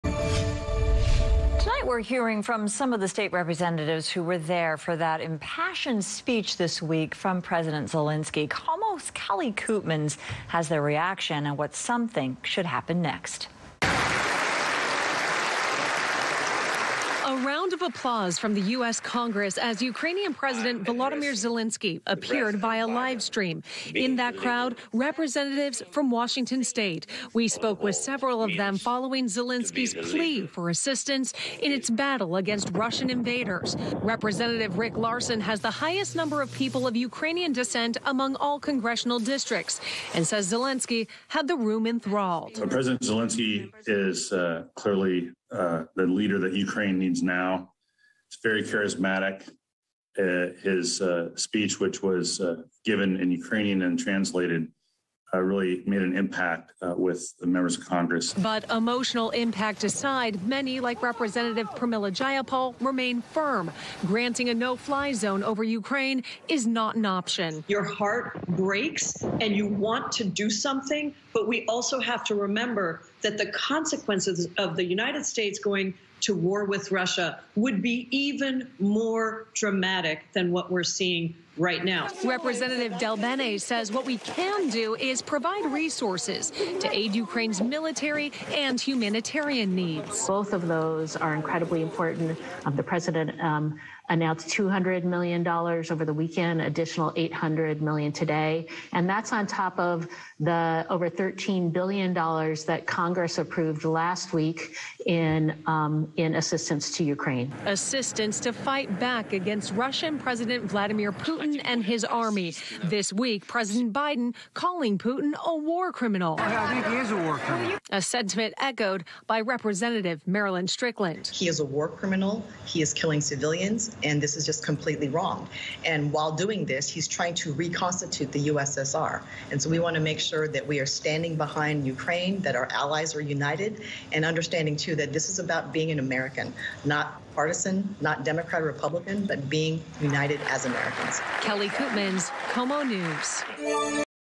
Rep. Strickland on KOMO: Putin is a War Criminal
On Saturday, March 19, 2022, Congresswoman Marilyn Strickland appeared on KOMO to discuss her reaction to President Zelensky's speech to Congress. During the interview, Congresswoman Strickland called Putin a war criminal for his illegal invasion of Ukraine and attacks on civilian targets.